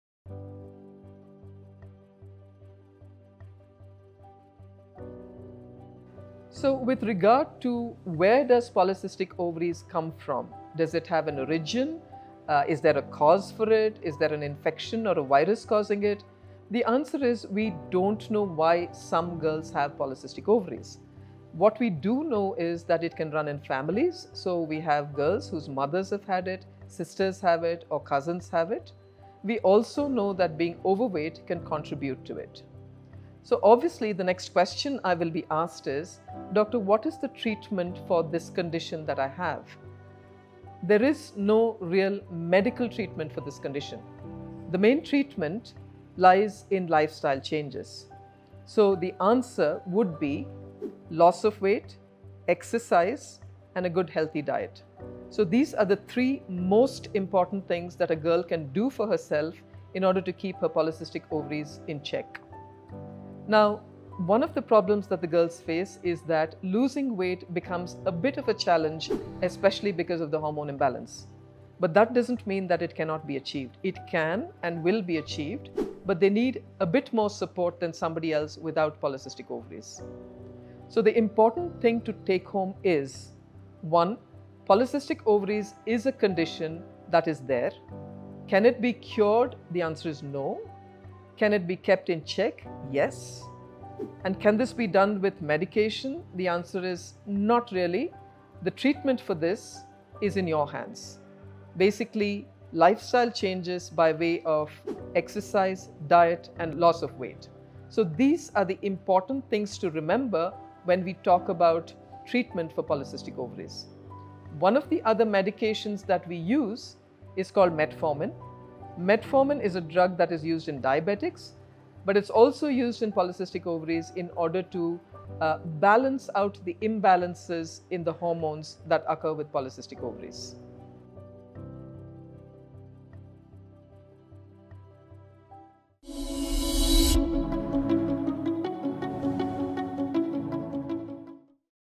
What are the treatment options for PCOS? | Dr. Prathima Reddy | SPARSH Hospital for Women & Children
Dr. Prathima Reddy, Director & Lead Consultant in the Department of Obstetrics and Gynaecology at SPARSH Hospital for Women & Children, Infantry Road, highlights that treatment for PCOS lies in your hands. Lifestyle changes such as weight loss, exercise, and a healthy diet can help you keep your condition in check.

This PCOSAwarenessMonth, let's spread awareness and knowledge about PCOS! Share your thoughts and experiences in the comments below. Together, we can empower women to take control of their health!